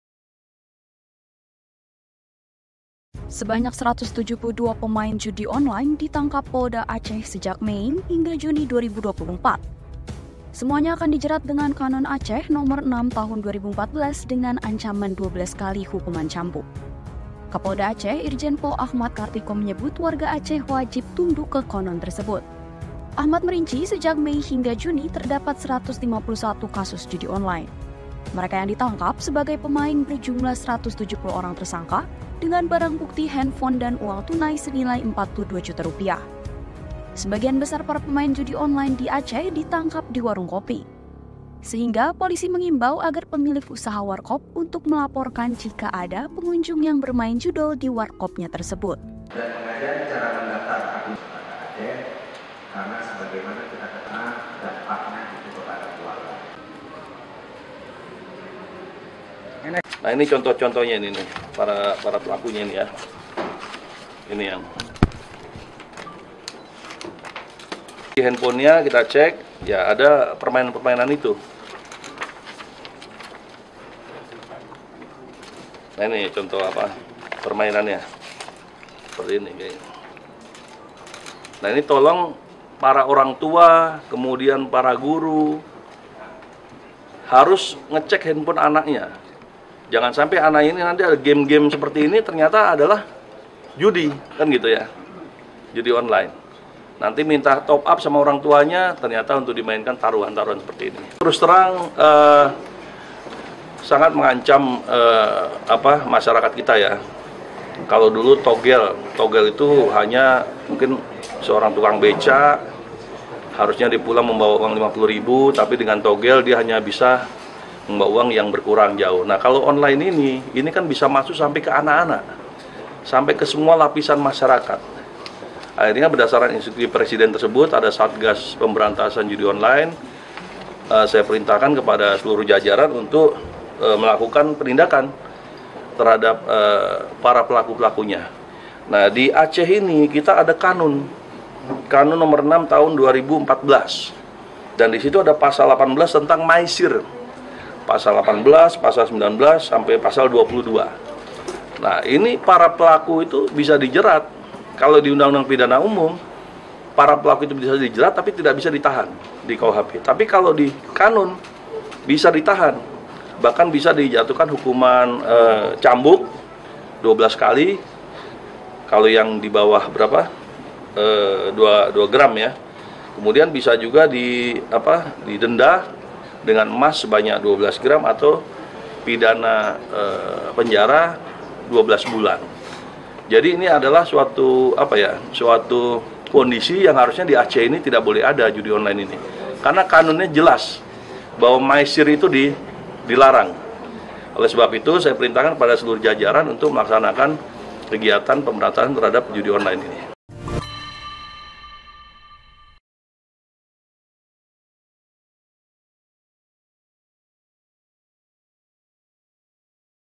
Polda Aceh Tangkap 172 Pelaku Judol, Dijerat Hukuman Cambuk
Sebanyak 172 pemain judi online ditangkap Polda Aceh sejak Mei hingga Juni 2024. Semuanya akan dijerat dengan Qanun Aceh Nomor 6 Tahun 2014 dengan ancaman 12 kali hukuman cambuk. Kapolda Aceh Irjen Pol Achmad Kartiko menyebut, warga Aceh wajib tunduk ke qanun tersebut. Achmad merinci sejak Mei-Juni terdapat 151 kasus judi online.

Mereka yang ditangkap sebagai pemain berjumlah 172 orang tersangka dengan barang bukti handphone dan uang tunai senilai Rp42 juta. Sebagian besar para pemain judi online di Aceh di tangkap di warung kopi. Sehingga, polisi mengimbau agar pemiliki usaha warkop untuk melaporkan jika ada pengunjung yang bermain judol di warkopnya tersebut.

Tanggal Tayang: 27Juni 2024